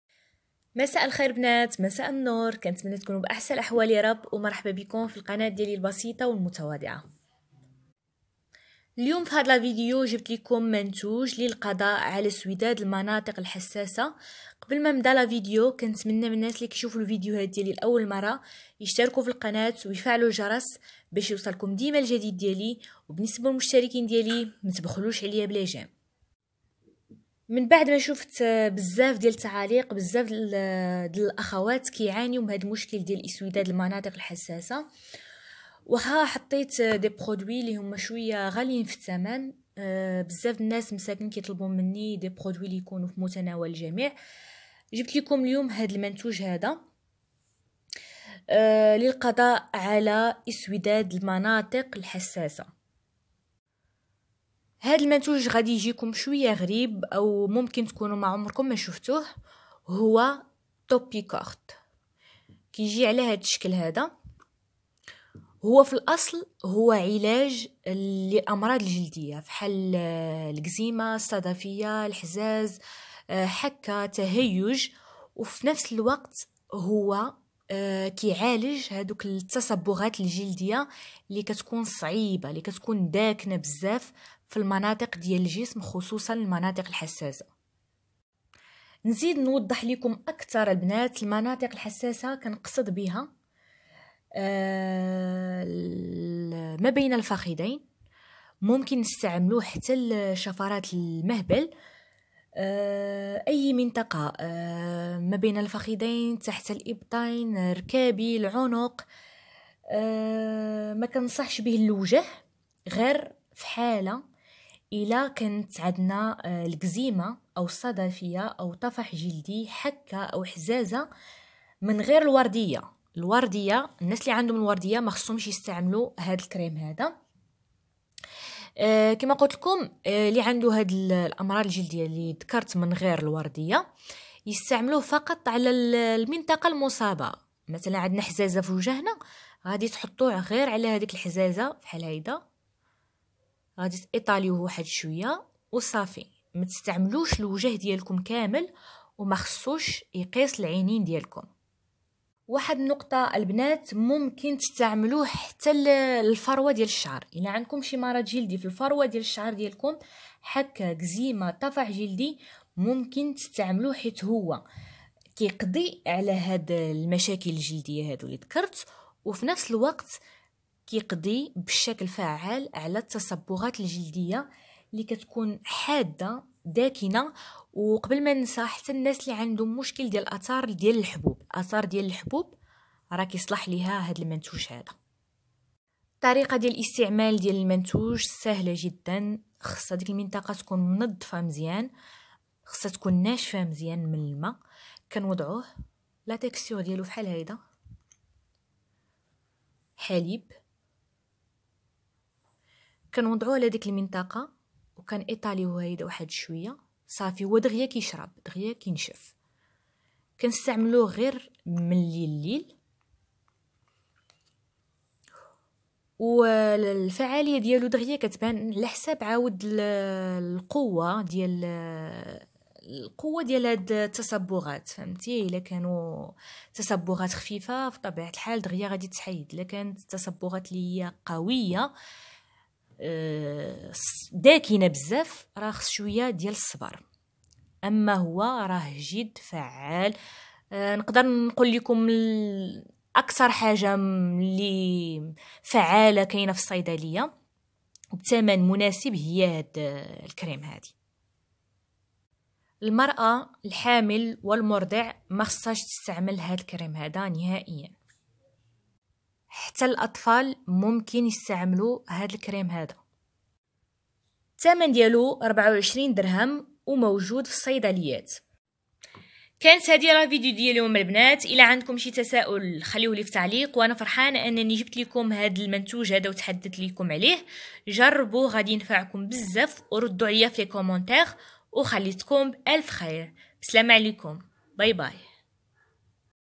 لتبييض المناطق الحساسة وشفرات المهبل منتوج غريب ب 24dh من الصيدليات,اسم المنتوج topicorte
topicorte